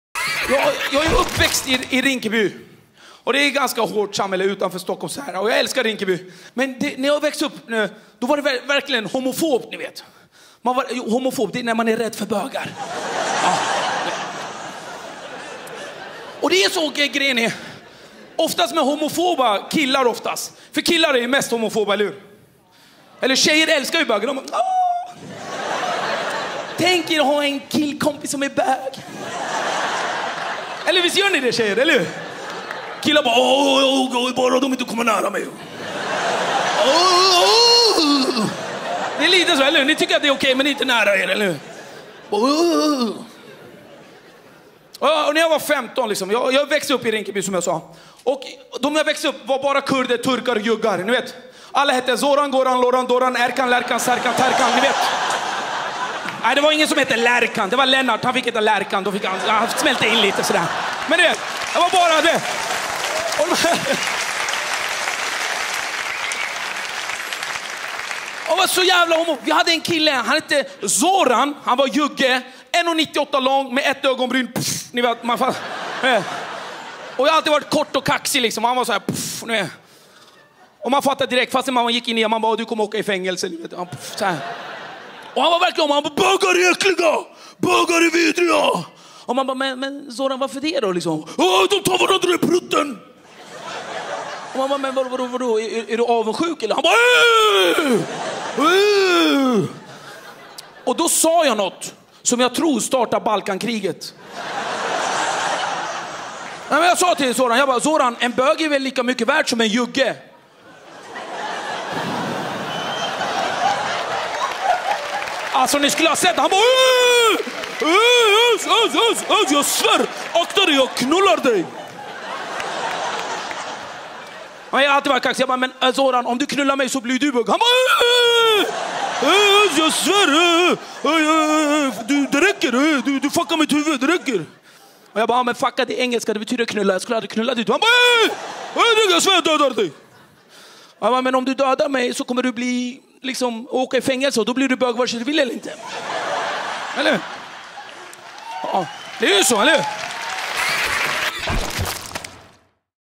Homofobi Özz Nujen
Özz pratar om sin uppväxt med homofobi i Rinkeby.